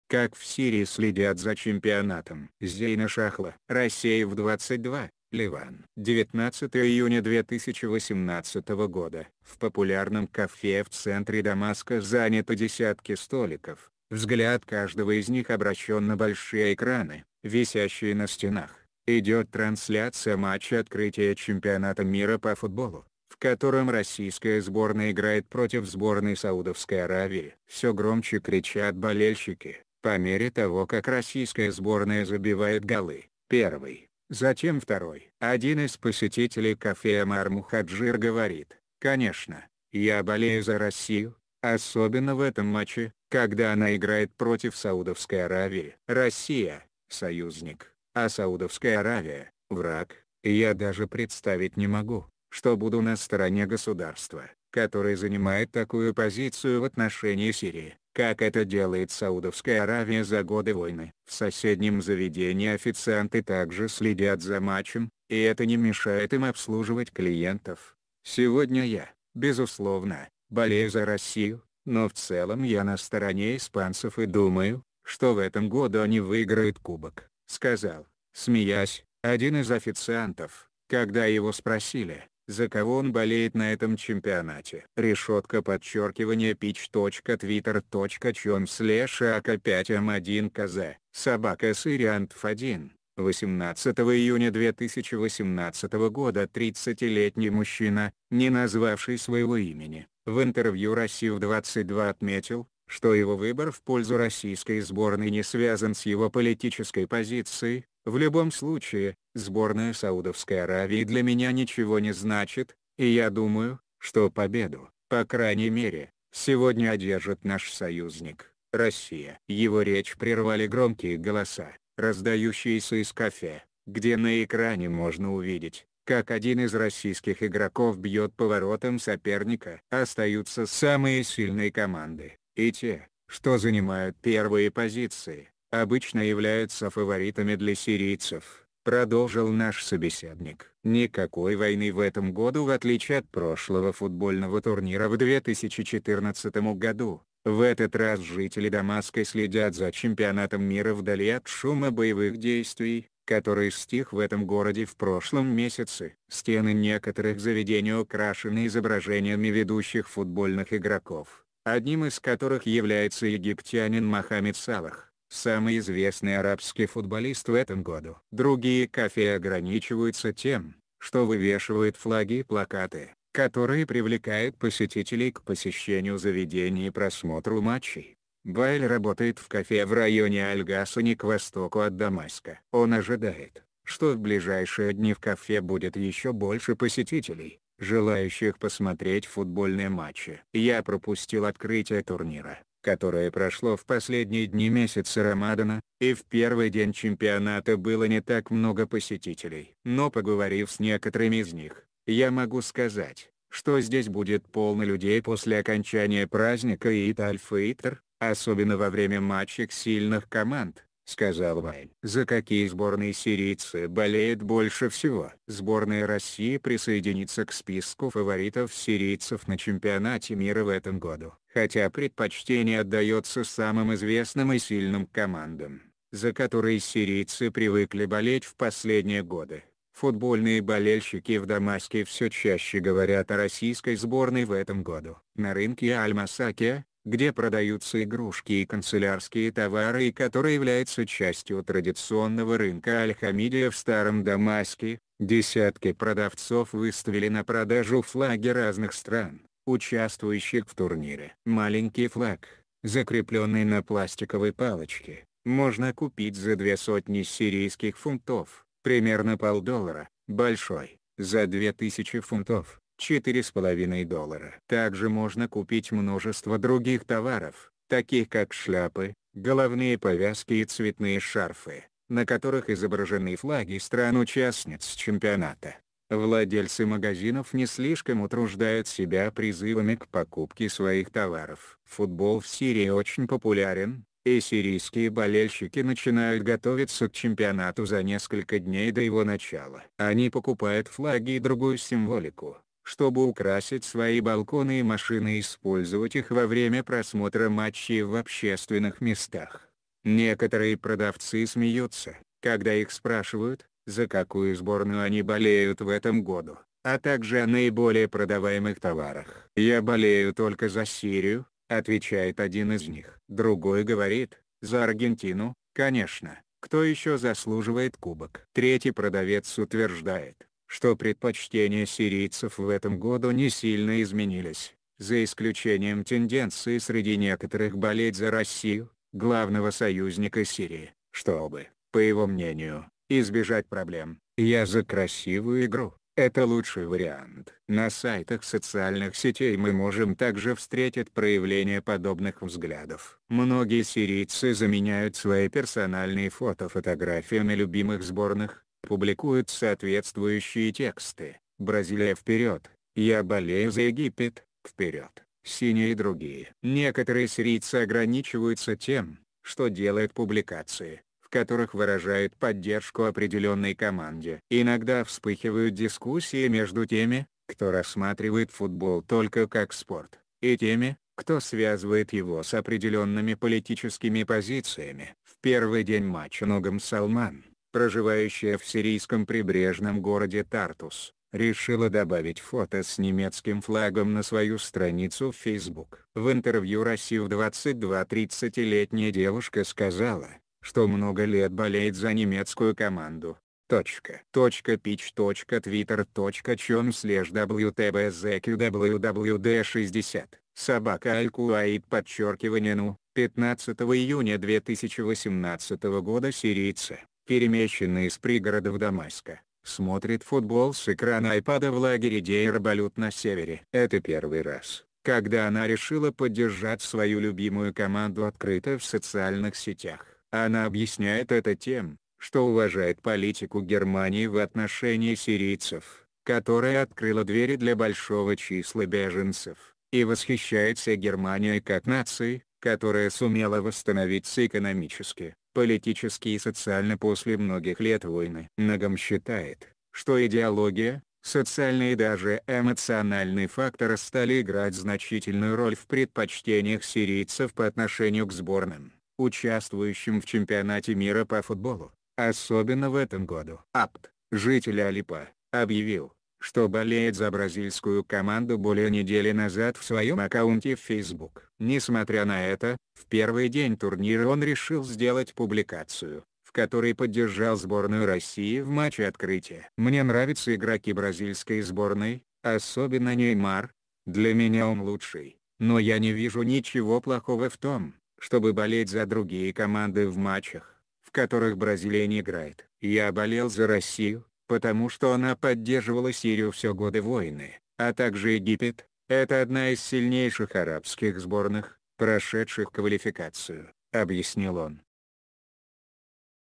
Как в Сирии следят за Чемпионатом. Raseef22, Ливан.
Как в Сирии следят за Чемпионатом.
Raseef22, Ливан.
Зейна Шахла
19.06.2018

В популярном кафе в центре Дамаска заняты десятки столиков, взгляд каждого из них обращён на большие экраны, висящие на стенах: идёт трансляция матча открытия Чемпионата мира по футболу, в котором российская сборная играет против сборной Саудовской Аравии. Все громче кричат болельщики, по мере того как российская сборная забивает голы: первый, затем второй… Один из посетителей кафе Омар Мухаджир говорит: «Конечно, я болею за Россию, особенно в этом матче, когда она играет против Саудовской Аравии. Россия — союзник, а Саудовская Аравия — враг, и я даже представить не могу, что буду на стороне государства, которое занимает такую позицию в отношении Сирии, как это делает Саудовская Аравия за годы войны». В соседнем заведении официанты также следят за матчем, и это не мешает им обслуживать клиентов. «Сегодня я, безусловно, болею за Россию, но в целом я на стороне испанцев и думаю, что в этом году они выиграют кубок», — сказал, смеясь, один из официантов, когда его спросили, за кого он болеет на этом чемпионате. أجواء مباريات كأس العالم من العاصمة دمشق تونس_انجلترا بالتوفيق لنسور قرطاج — حكاية وطن سورية (@syriantv1) 18 июня 2018 г. ​ 30-летний мужчина, не назвавший своего имени, в интервью «Расиф22» отметил, что его выбор в пользу российской сборной не связан с его политической позицией: «В любом случае, сборная Саудовской Аравии для меня ничего не значит, и я думаю, что победу, по крайней мере, сегодня одержит наш союзник — Россия». Его ре...